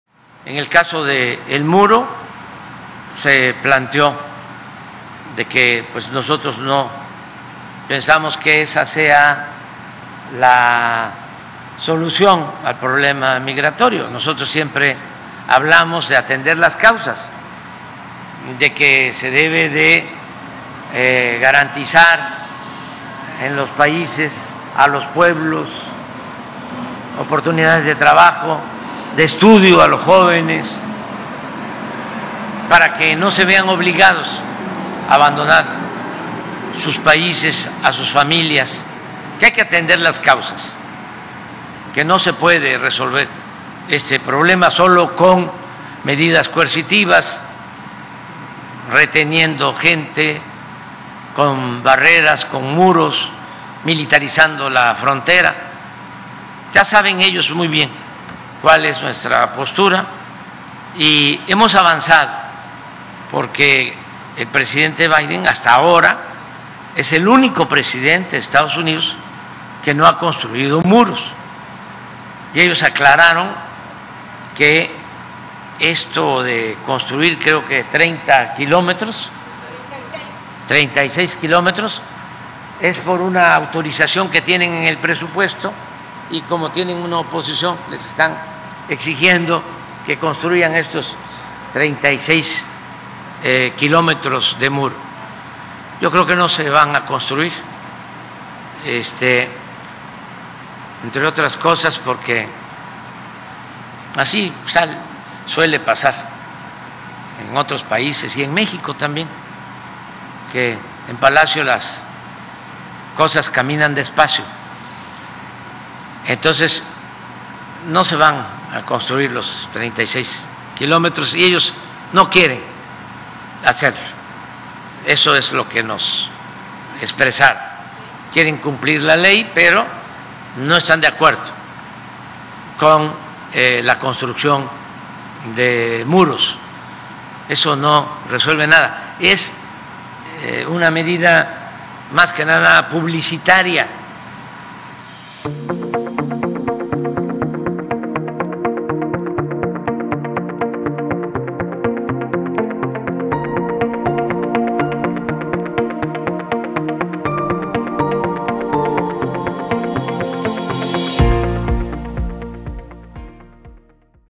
El Presidente dice que los enviados de Biden ven el muro como “medida publicitaria”
El Presidente Andrés Manuel López Obrador aseguró este viernes que la delegación del Gobierno de Estados Unidos, con la que se reunió ayer, le garantizó que no se construirán los 36 kilómetros de muro en la frontera y reiteró que se trata de una “medida publicitaria” de un sector ultraconservador de ese país.